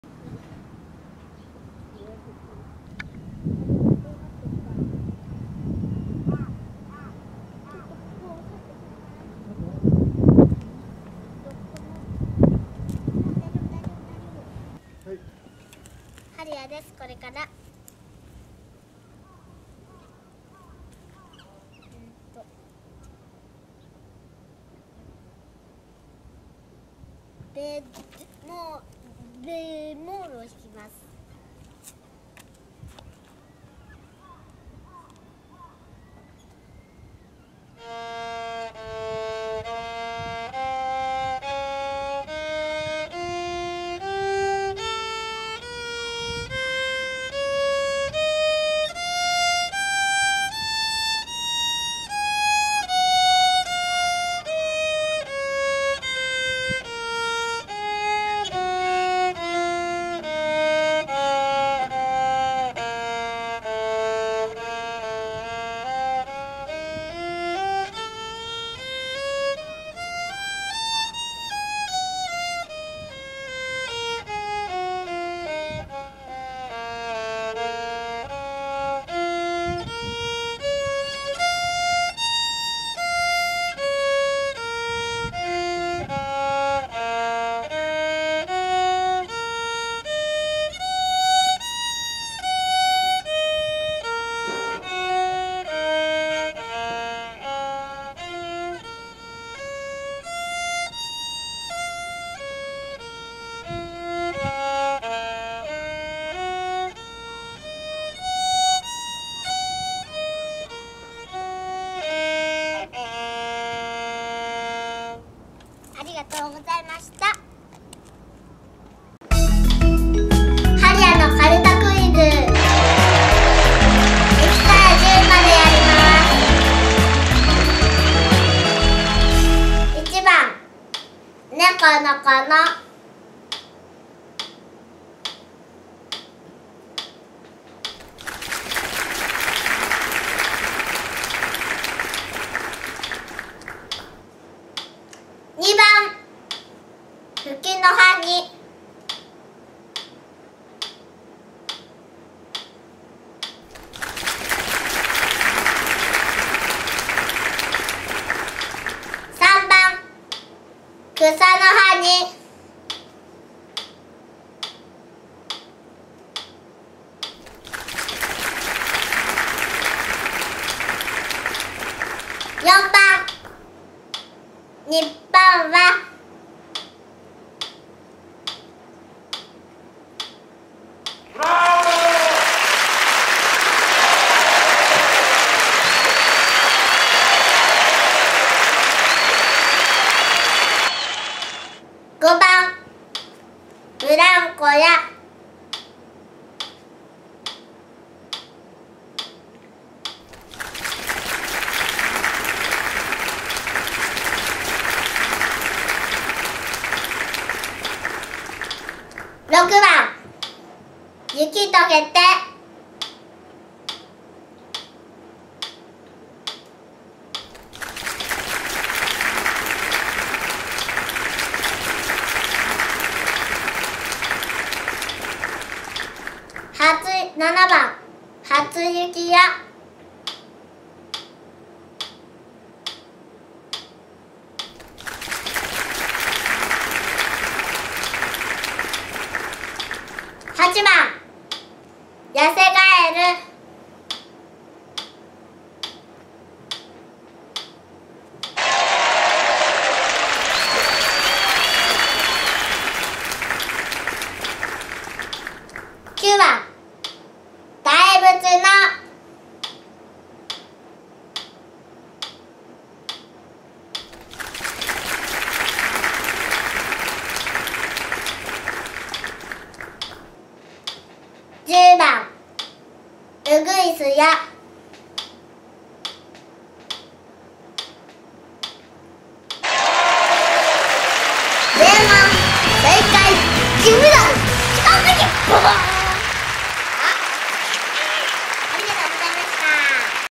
Scale - b-moll (B-flat minor) in 1st position [変ロ短調の音階]
♪音階練習ツアー♪　[ scale practice tour !? ]　小野アンナ音階教本より「変ロ短調の音階」
Location パパの母校の小学校裏にて。
バイオリン：　はるや（6才）